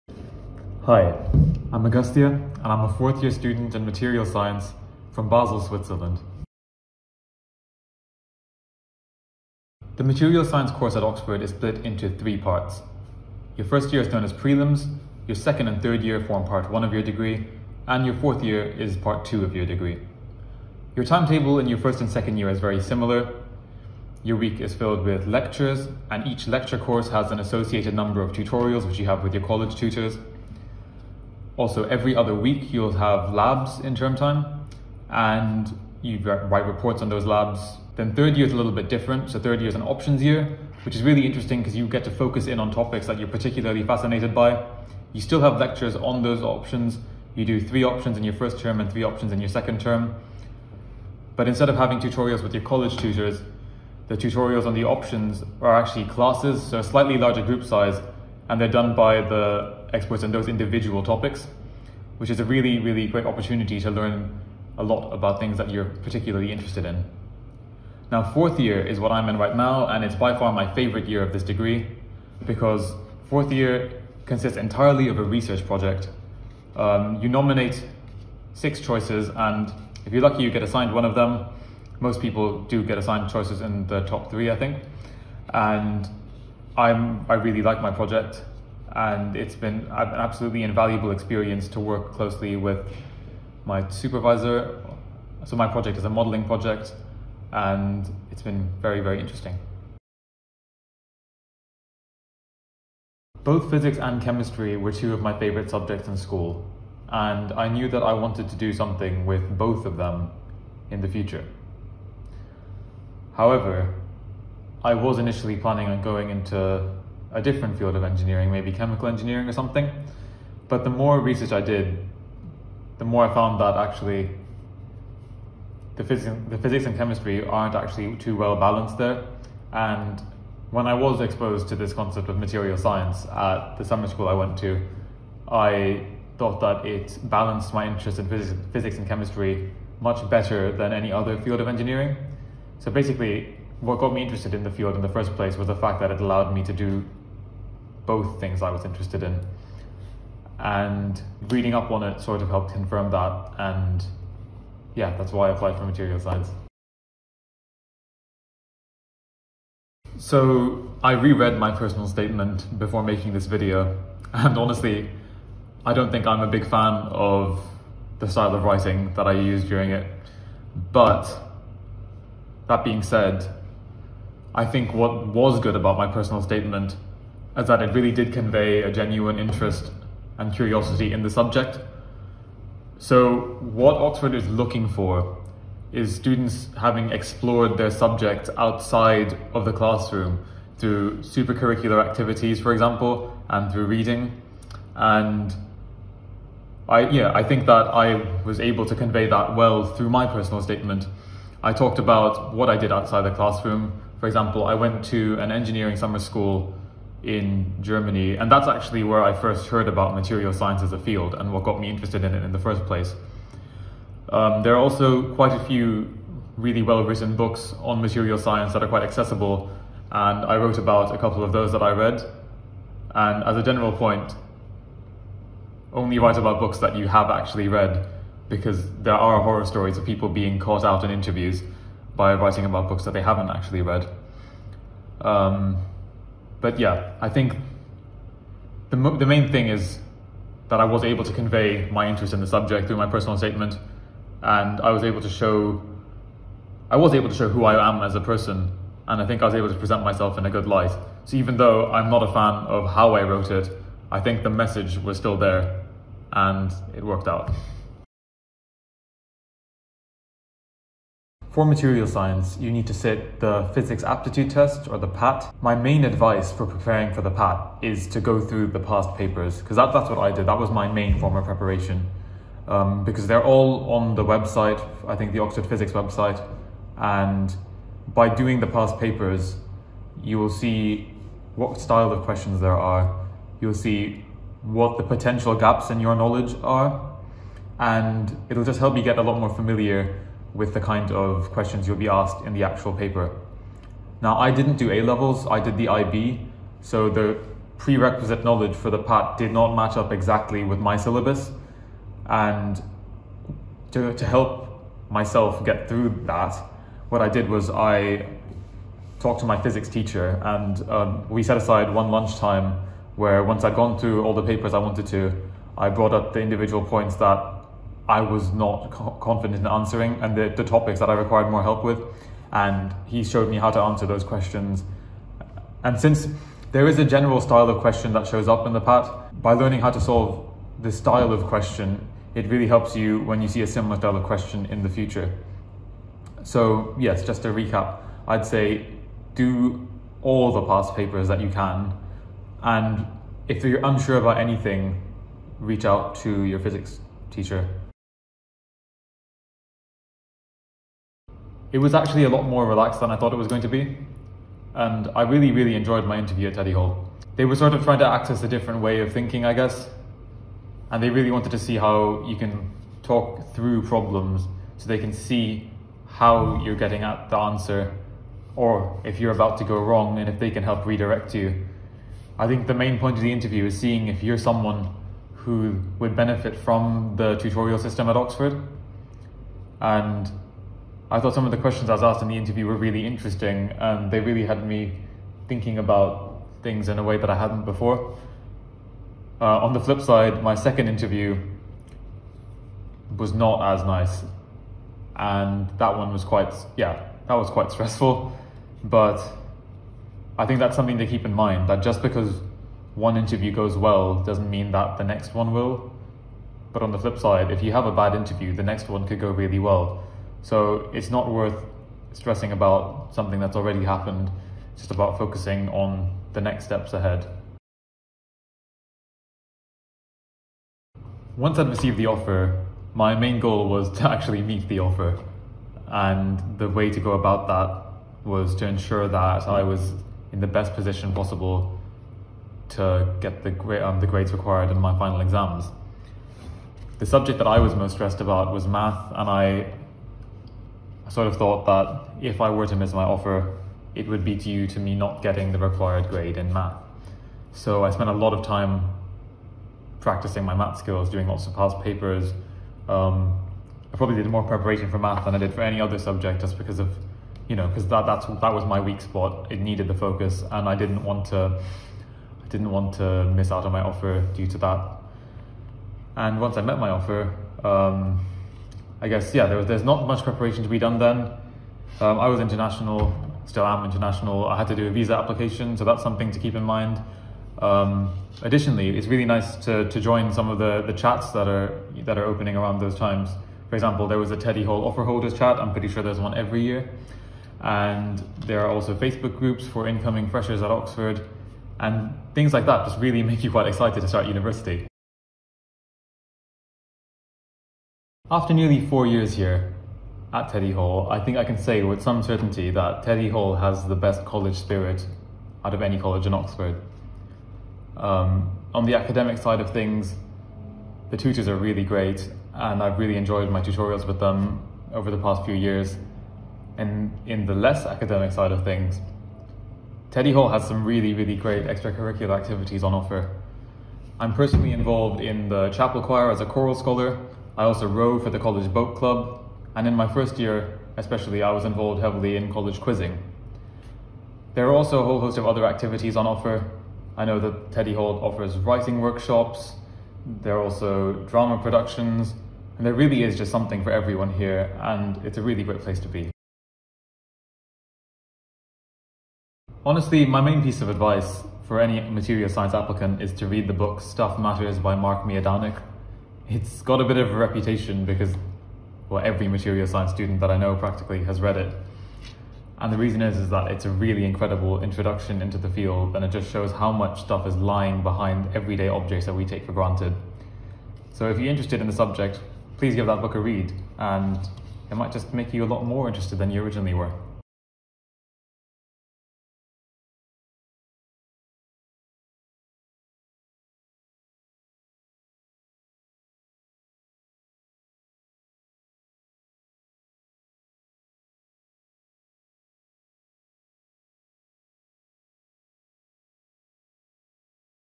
Q&A with an Oxford Materials Science Student at St Edmund Hall
Teddy Hall student Agastya answers questions on applying to Materials Science at Oxford.
Ask your own questions in the comments section below!

The book that Agastya refers to at 09:10 is Stuff Matters by Mark Miodownik

00:00 Introduction
00:09 What's your course like?
01:42 What made you choose Materials Science?
02:39 What was the best part of your Personal Statement?
04:20 How did you prepare for the Admissions Test?
05:50 What was the interview like?
06:59 What preparation did you do after receiving an offer?
08:16 What is it like studying at Teddy Hall?
09:09 Do you have any advice for someone applying to Materials Science at Oxford?